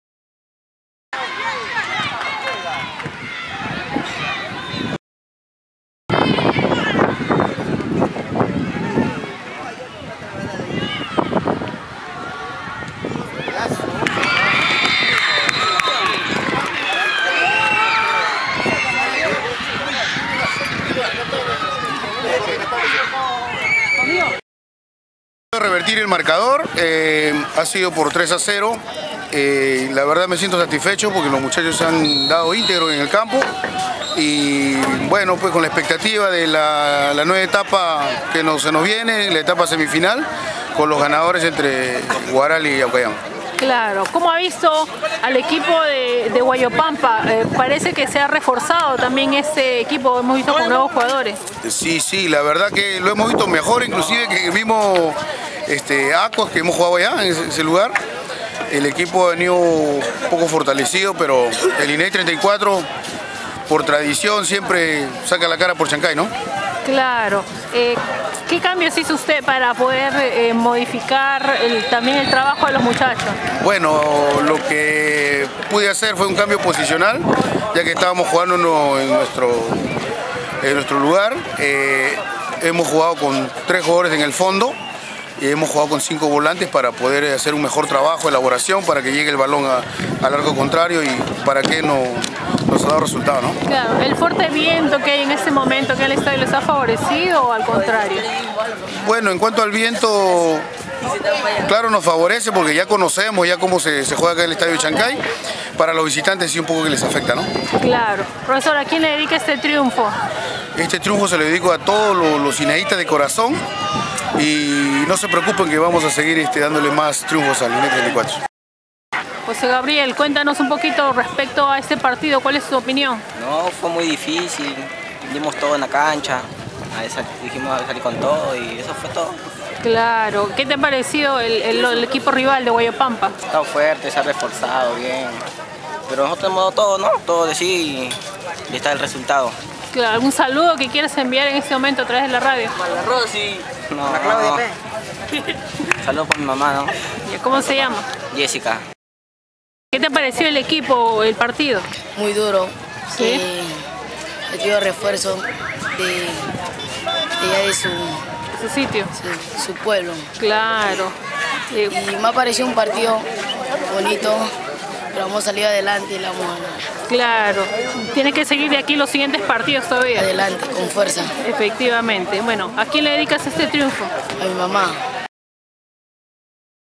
CHANCAY: INEI 3 - Huayopampa 0 sub 14
08 julio 2014.- Publicamos un gol de tres que hizo la selección de  fútbol del colegio INEI 34 de Chancay frente a Huayopampa, Declaraciones del entrenador Elio Nario y de dos jugadores de la sub 14.